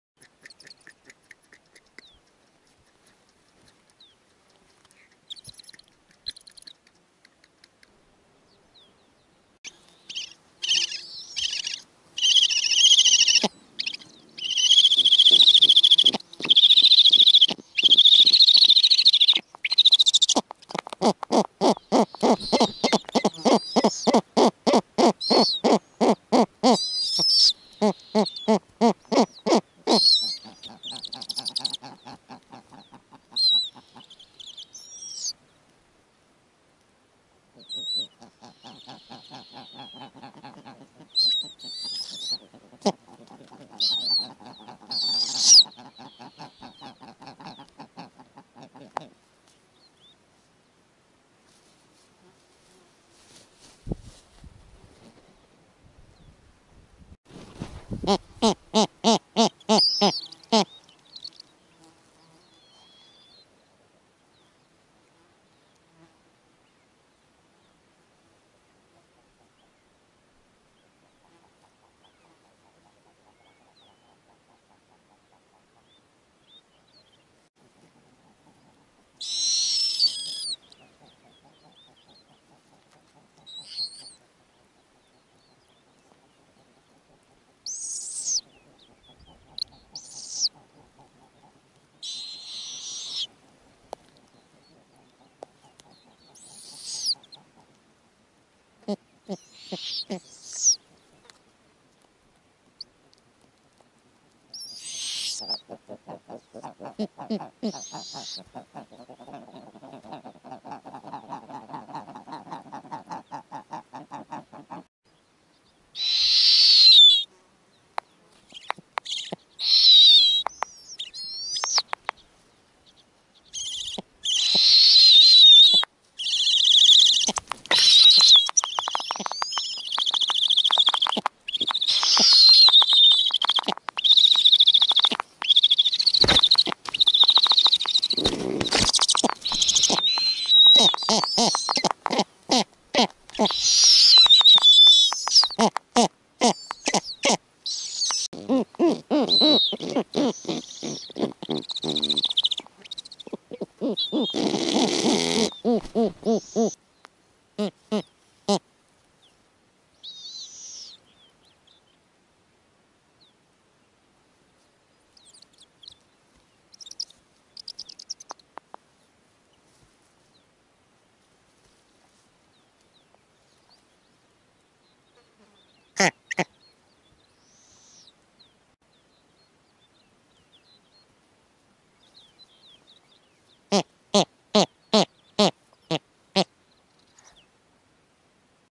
Snowy owls: What is it like to raise 8 multi-age chicks?
Snowy owls are an iconic species of the Arctic. Animal world of the Arctic. Birds of prey. Snowy owl on the nest, parenting behavior, animal sounds.
Snowy owl (named Didi) nest. Summer time in the Arctic.  Didi laid 9 eggs in spring. Now she has 8 owlets of different ages. The younger ones are still small, helpless and blind - the female must warm them. The older ones are already quite independent for thermoregulation and can themselves warm the younger ones.
Owls begin incubation by laying their first egg. Therefore, their owlets can have a big difference in age and size. In this nest, the older one hatched 18 days earlier than the younger one and has already eaten several dozen lemmings. Parents are very busy catching prey and feeding offspring.